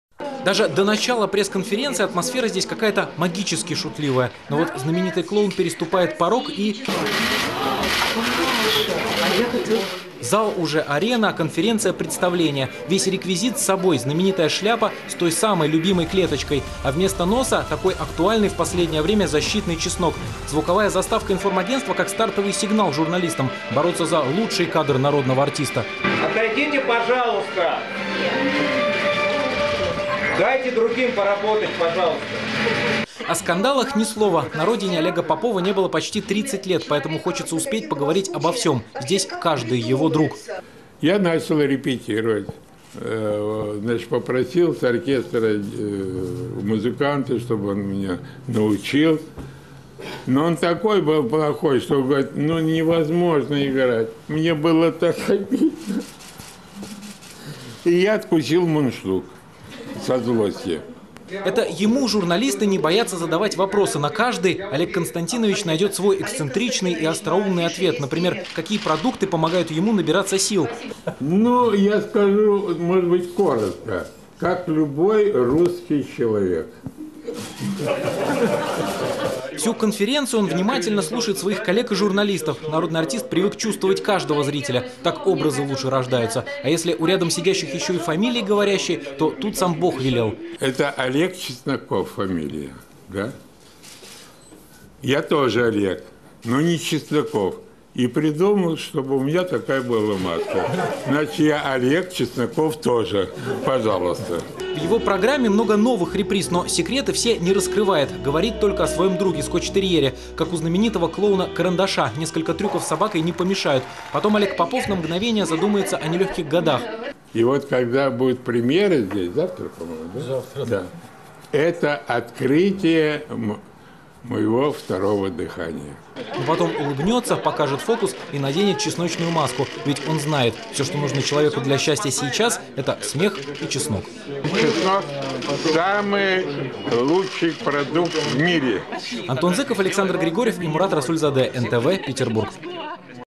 Тема гриппа раскрыта (11.02.2016)
Олег Попов в чесночной маске одаривал питерских журналистов эксцентричными шутками.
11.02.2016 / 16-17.

Олег Попов и организаторы его петербургских гастролей дают пресс-конференцию. Первые комментарии клоуна о финансовом скандале.